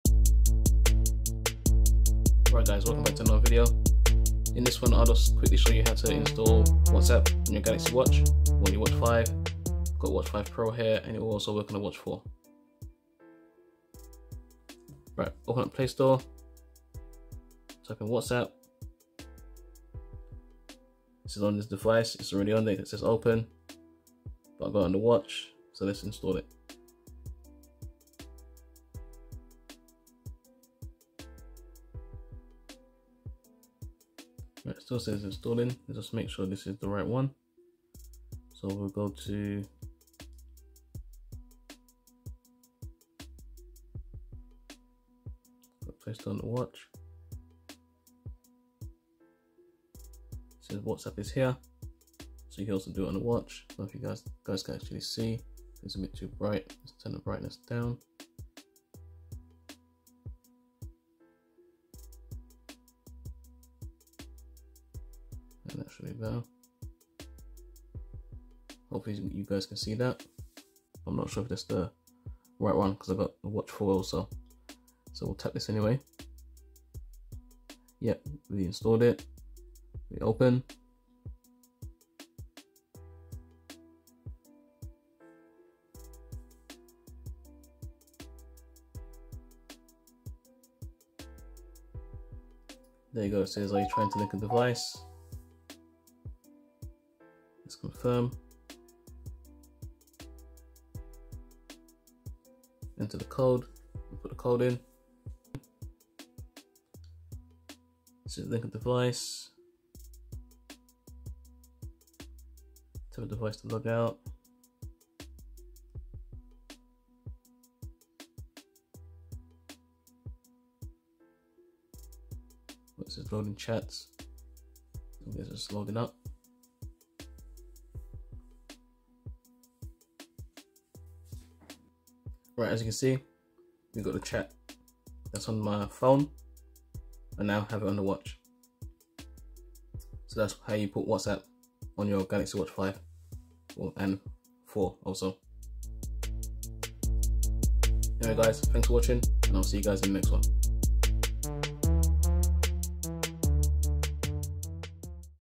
How To Install WhatsApp On Your Samsung Galaxy Watch 5/4
This is how to install WhatsApp easily on your Galaxy Watch 5 and 4